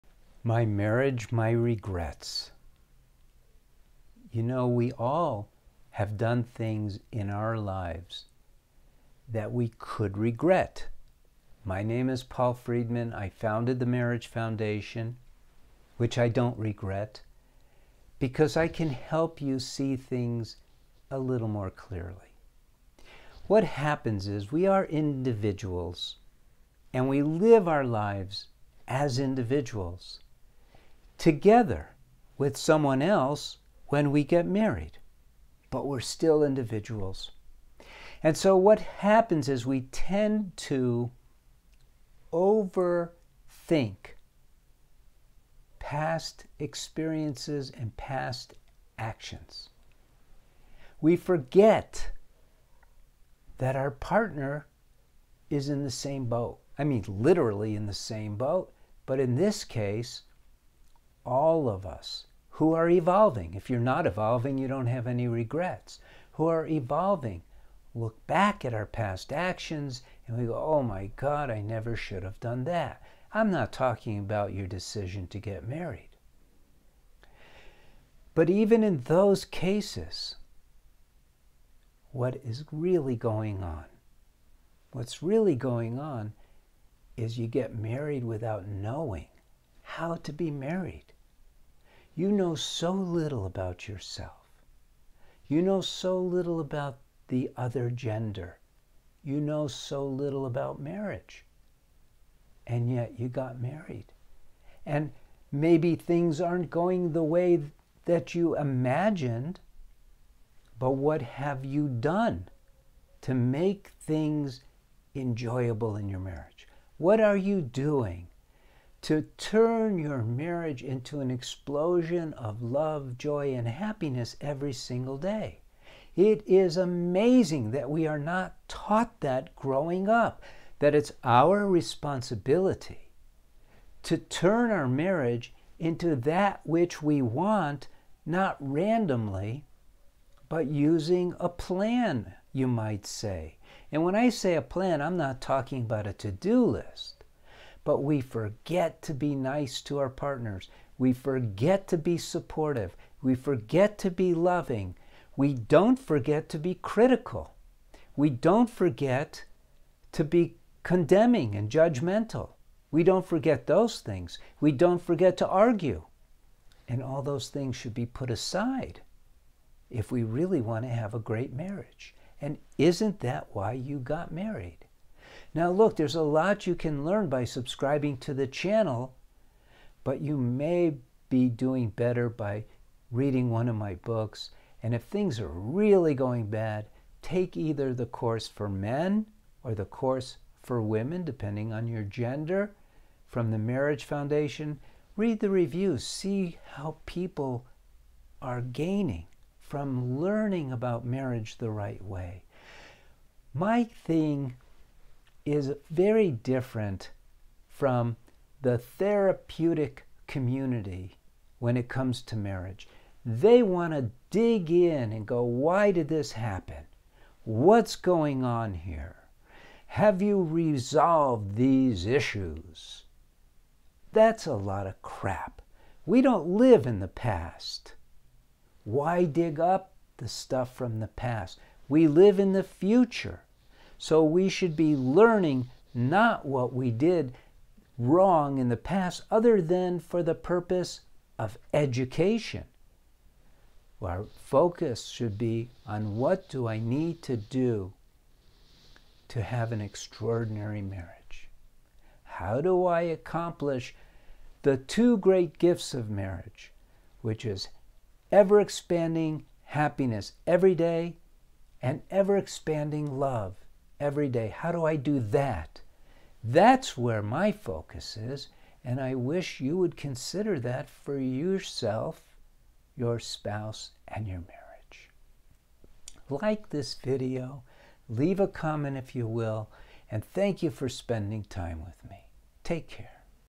"My Marriage, My Regrets" | Paul Friedman
We all have done things in our lives that we could regret. What happens is we are individuals and we live our lives as individuals together with someone else when we get married but we're still individuals. And so what happens is we tend to overthink past experiences and past actions. We forget that our partner is in the same boat. I mean literally in the same boat but in this case, all of us who are evolving if you're not evolving, you don't have any regrets, who are evolving look back at our past actions and we go, "Oh my God! I never should have done that." I'm not talking about your decision to get married but even in those cases, what is really going on, what's really going on is you get married without knowing how to be married. You know so little about yourself. You know so little about the other gender. You know so little about marriage and yet, you got married and maybe things aren't going the way that you imagined but what have you done to make things enjoyable in your marriage. What are you doing to turn your marriage into an explosion of love, joy, and happiness every single day?